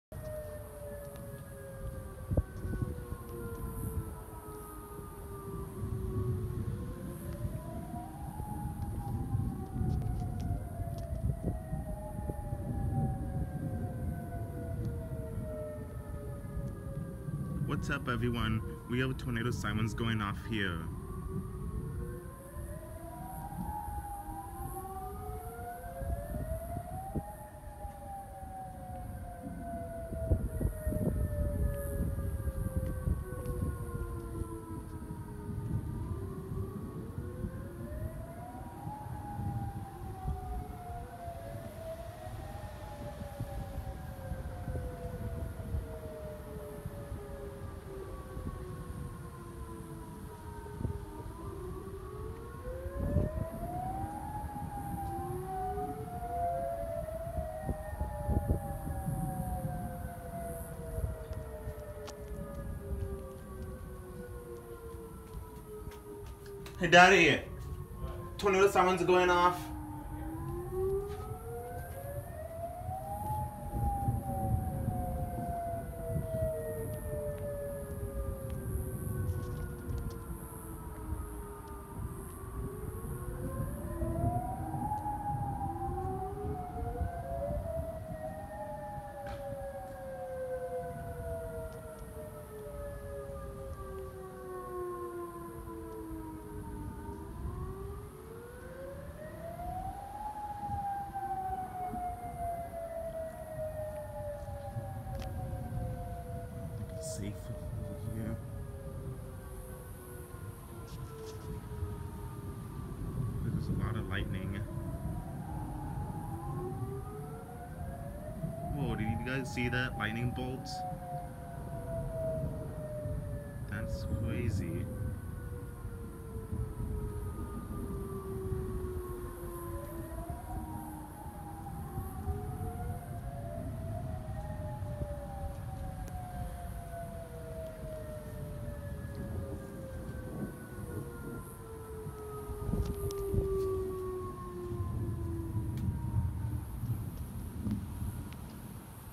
Tornado Warning in Allen, TX 3/26/17
The Tornado syrens are going off here in Allen, TX at around 8:30 pm on March 26, 2017 and those of you people near my location, hope you guys are okay, make sure to like and subscribe